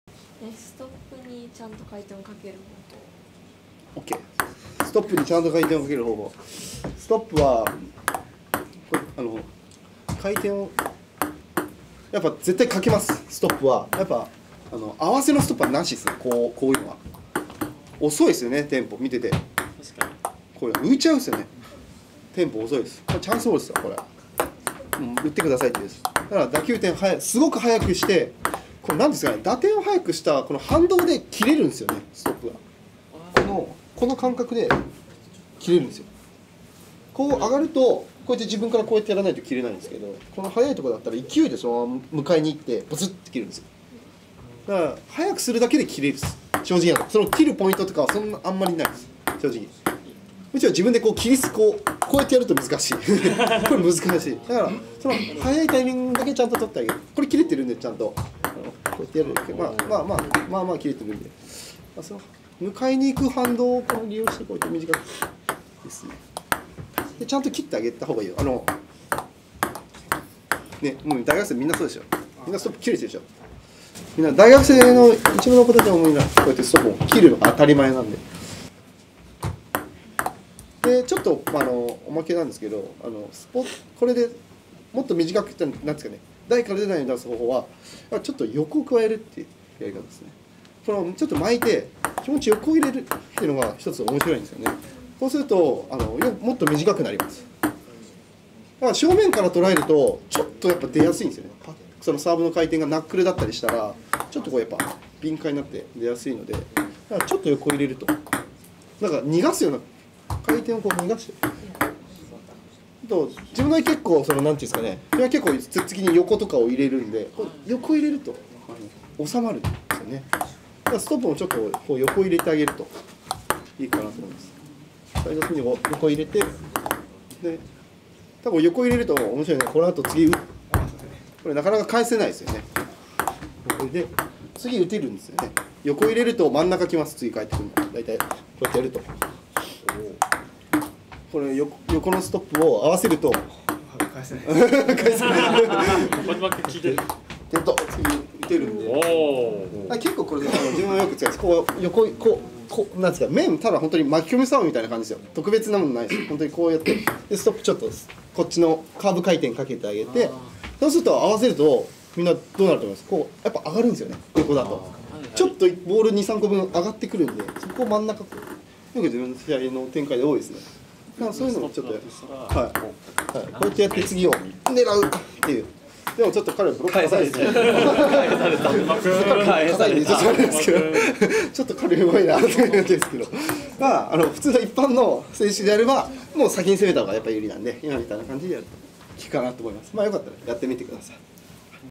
ストップ＆サイドスピンストップ講座-ぐっちぃ正月講習会【卓球知恵袋】
1月3日にやった正月講習会の模様です。内容は『ストップ＆サイドスピンストップ講座』です！動画の前半は、ストップでどうやって回転をかけるか？ということについてやっています！後半はストップを短く止める方法についてやっています！　最後はぐっちぃおすすめの2球目ストップレシーブからほぼ確実に4球目で先手が取れるという実践的な展開も紹介しています！

相手をしてくれているのが小島くんです。マイクが遠いので音量が少し小さめになっています。講習の中身をしっかり聞くために、ヘッドホンか音量を高めにすることを推奨します。

全国にいるみなさんに少しでもお力になれるよう動画で公開してみました！